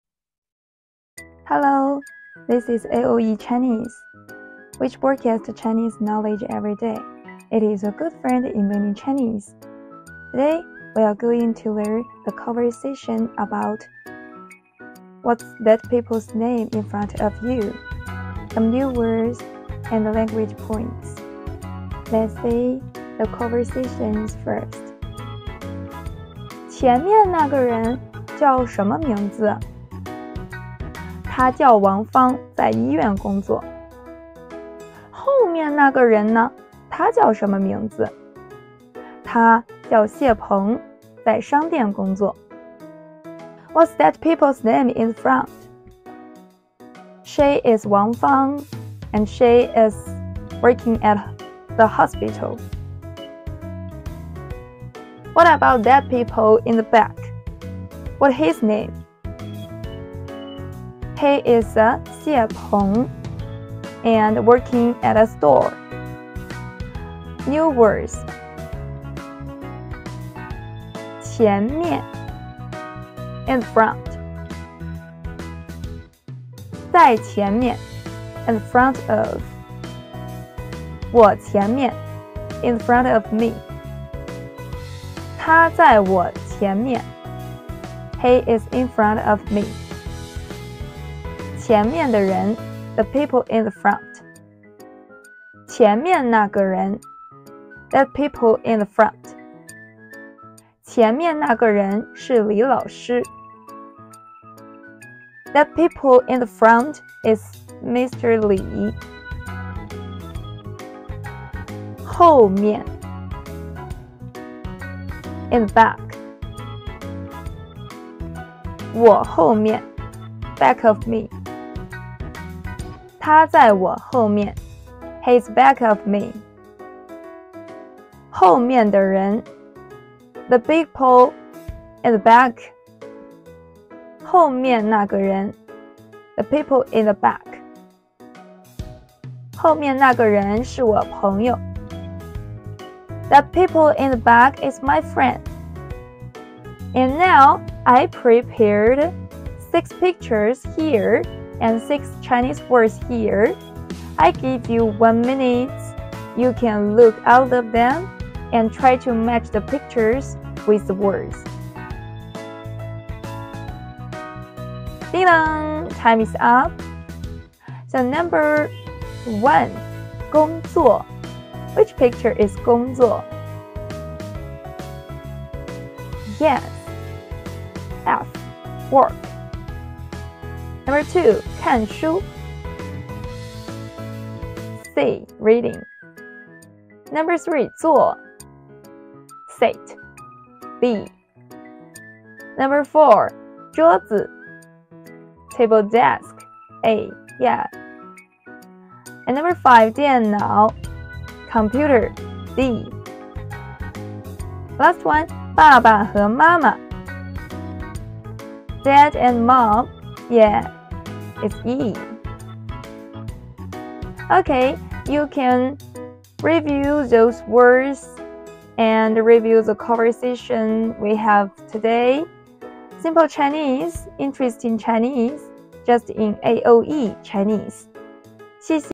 What's that person's name in front of you?|AoeChinese
Curious how to ask 'What's that person's name in front of you?' in Chinese? 🉐🗣️ Watch the video as we unravel this useful phrase and enhance our language skills together! 🌟